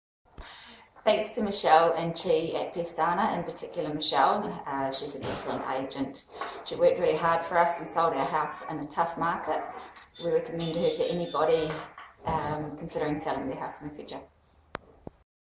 Pestana Realty Home Seller Testimonial # 38 - Chee Yap
Thanks Michelle and Chee Yap at Pestana Realty. In particular Michelle, she's an excellent agent. She worked hard for us and sold our house in a tough market. We recommend her to anybody considering selling their house in the future.